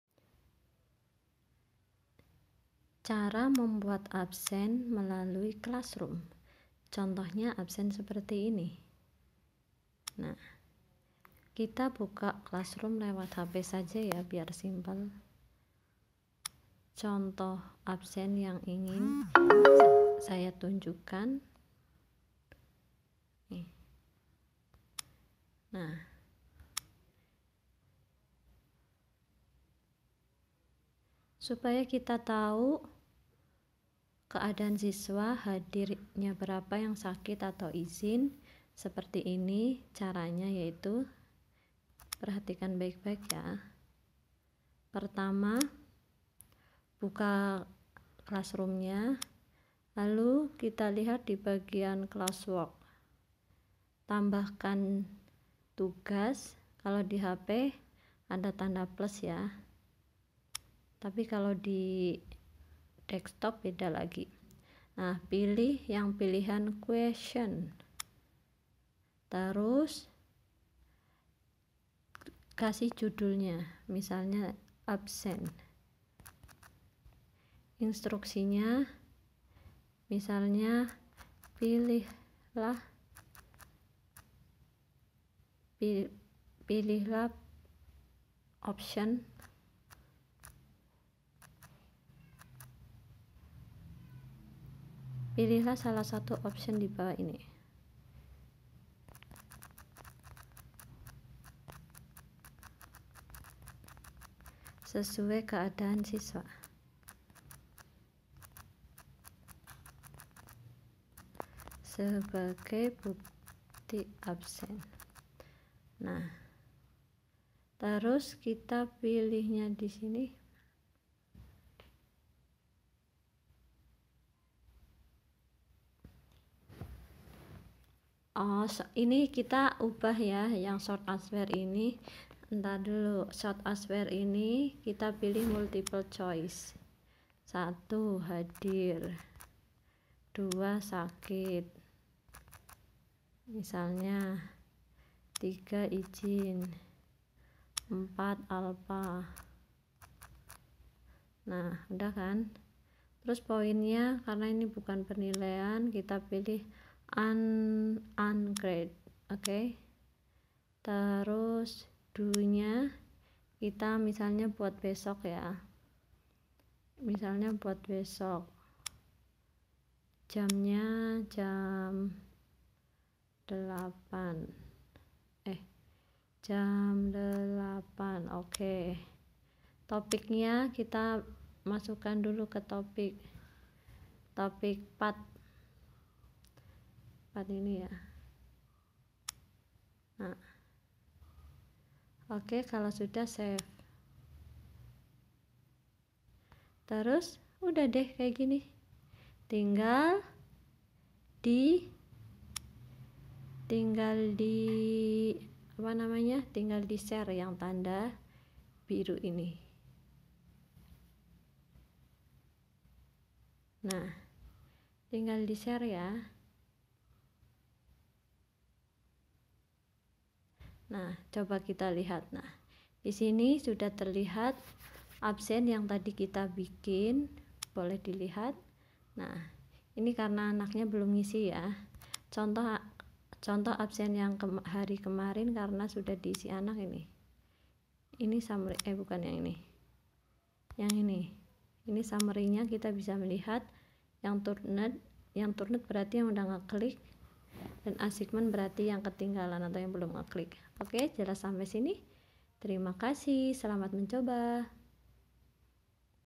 Cara membuat absen siswa di clasroom
Memuat tutorial membuat absensi siswa di clasroom melalui hp. Semoga video ini bisa bermanfaat untuk guru yang menggunakan clasroom dalam pembelajaran jarak jauh.